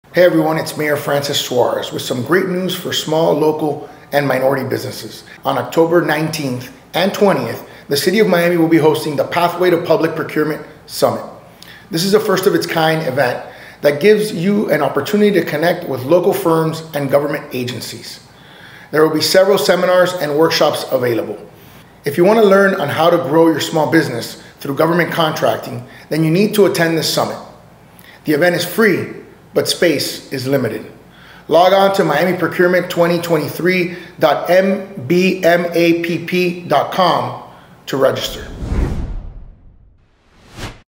Join us at the Pathways to Public Procurement Summit 2023!
Join us at the Pathways to Public Procurement Summit 2023! 🌟 Discover opportunities for small businesses in government contracting on October 19th and 20th. Secure your spot by registering at miamiprocurement2023.mbmapp.com.